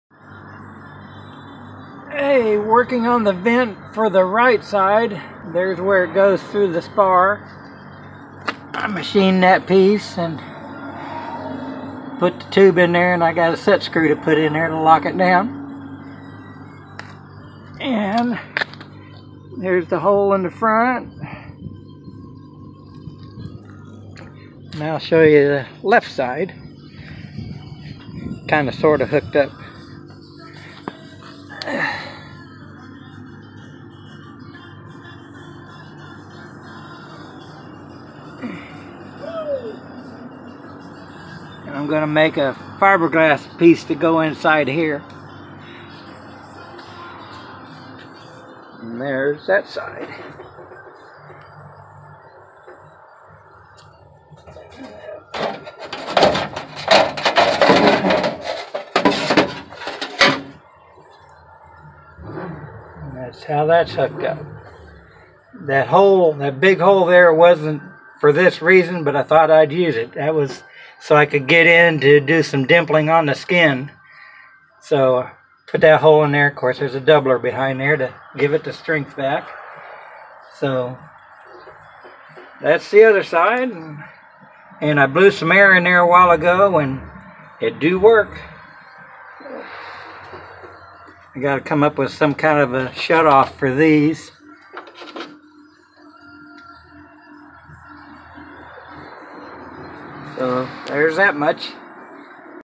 H5 secondary vents
I'm working on my H5 secondary vents. I machined the fittings to go though the spar web and got them riveted in. I'm going to mold the fittings out of fiberglass for the LE.